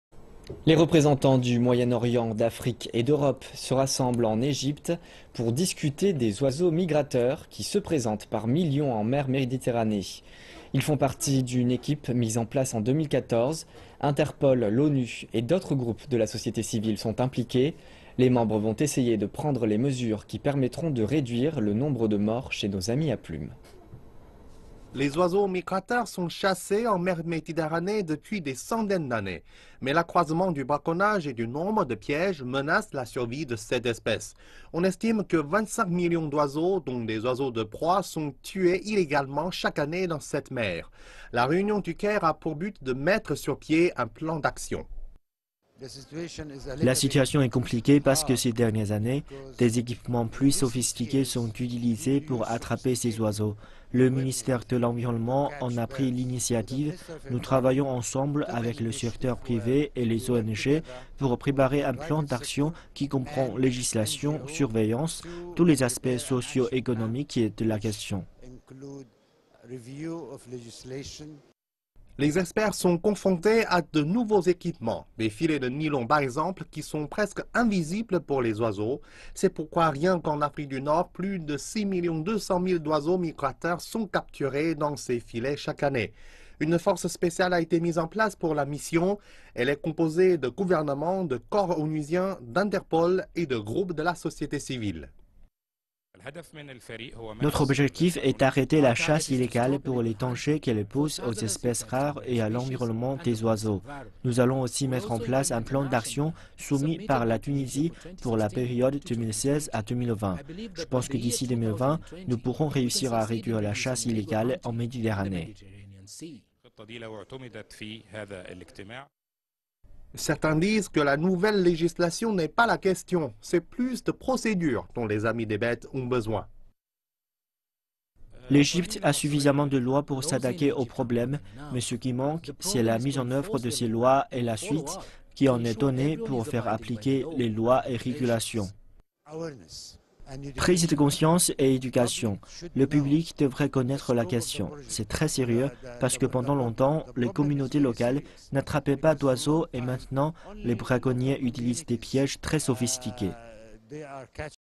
Une force spéciale pour protéger les oiseaux migrateurs
Les représentants du Moyen-Orient, d'Afrique et d'Europe se rassemblent en Egypte pour discuter des oiseaux migrateurs qui se présentent par millions en mer Méditerrannée.